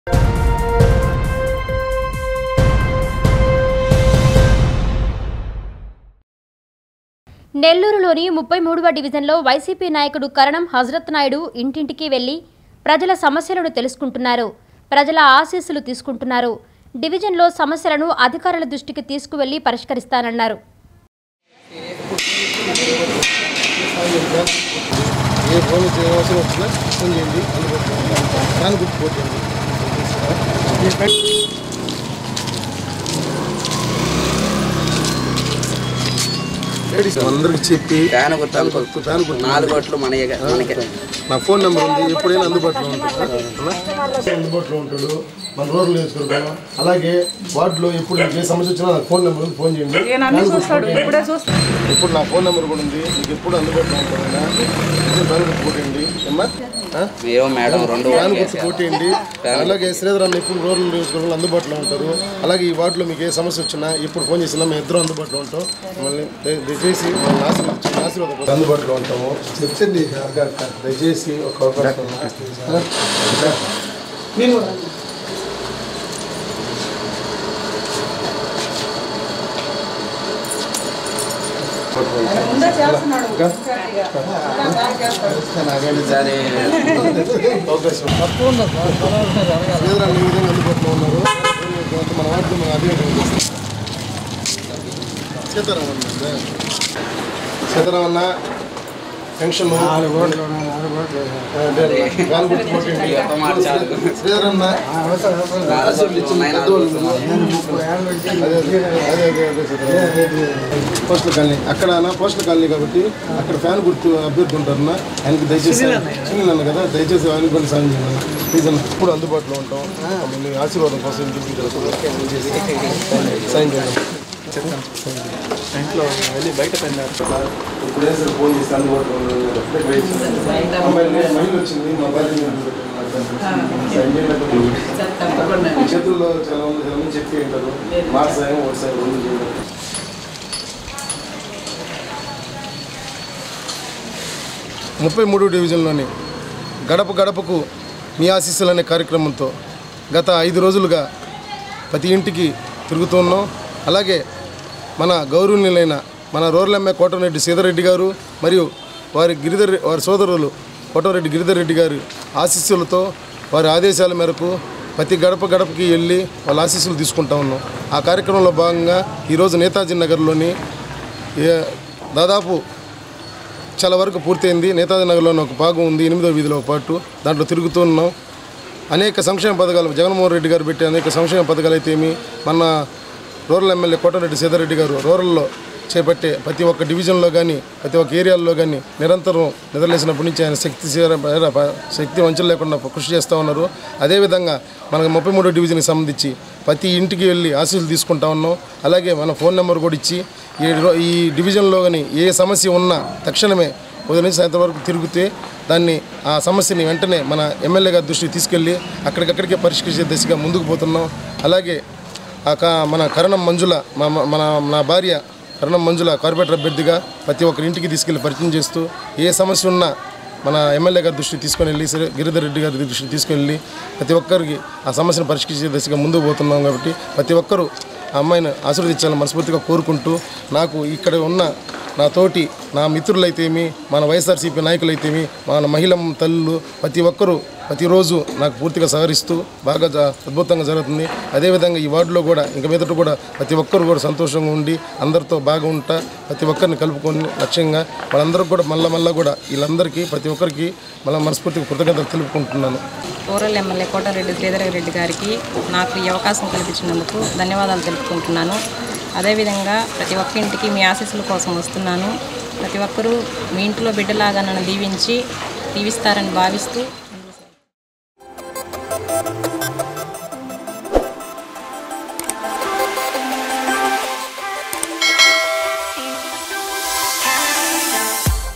నెల్లూరులోని 33వ డివిజన్లో ప్రజల ఆశీస్సులు.. - NDN News
నెల్లూరులోని 33వ డివిజన్లో వైసీపీ నాయకుడు కరణం హజరత్ నాయుడు ఇంటింటికీ వెళ్లి.. ప్రజల సమస్యలను తెలుసుకుంటున్నారు. ప్రజల ఆశీస్సులు తీసుకుంటున్నారు. డివిజన్లో సమస్యలను అధికారుల దృష్టికి తీసుకెళ్లి పరిష్కరిస్తానని చెప్పారు.